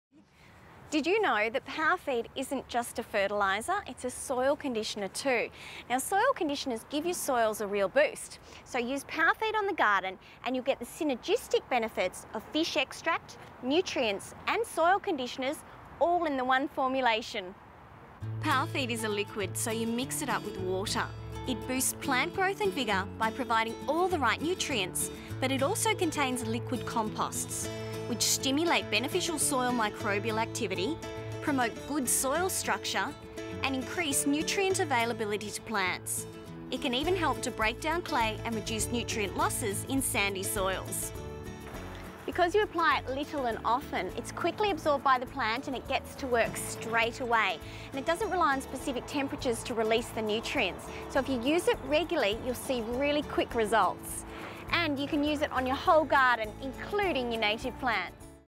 The Garden Gurus - Seasol Powerfeed Green
Using PowerFeed Green on your soil gives your garden the synergistic benefits of fish extract, nutrients and soil conditioners all in one formulation.